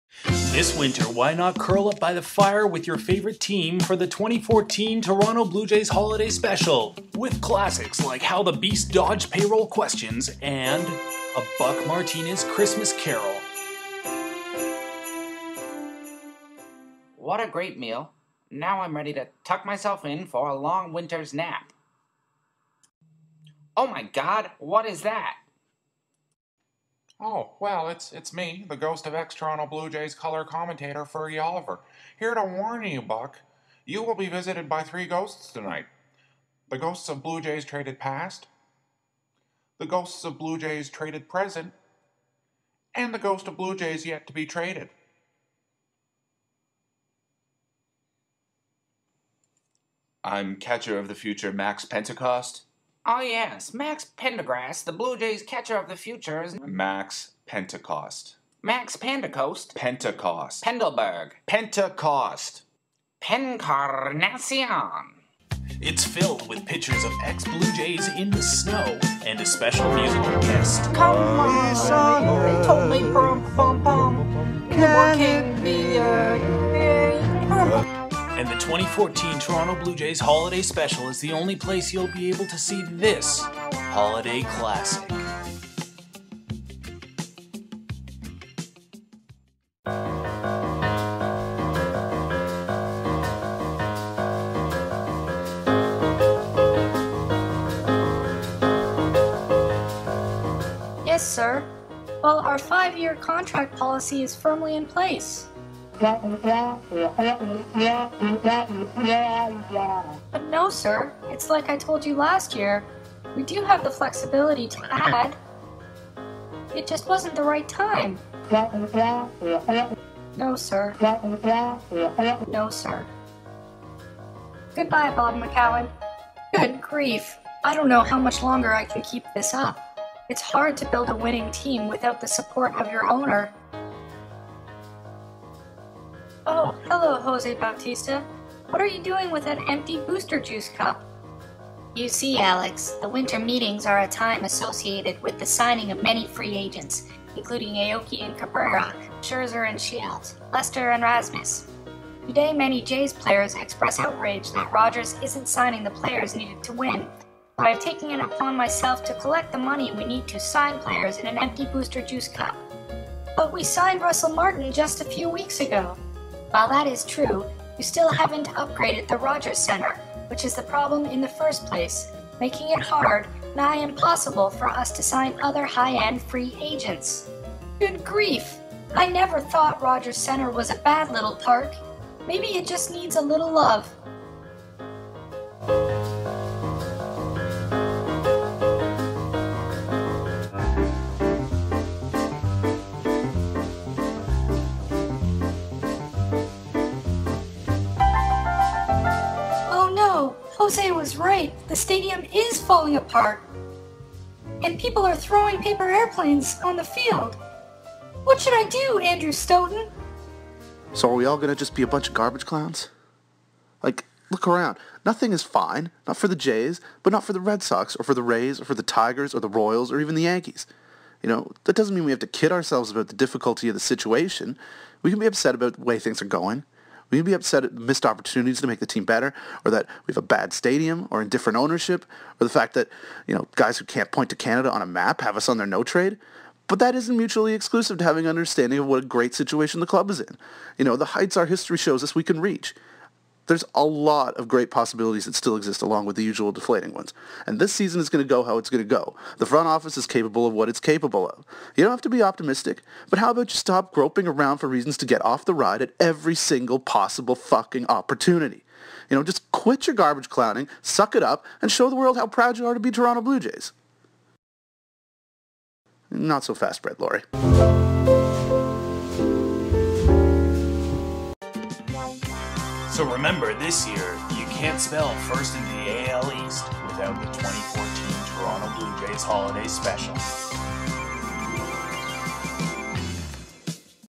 2014 Toronto Blue Jays Holiday Special
An extended commercial for the 2014 Toronto Blue Jays Holiday Special - because you can't spell "1st in da AL East" without it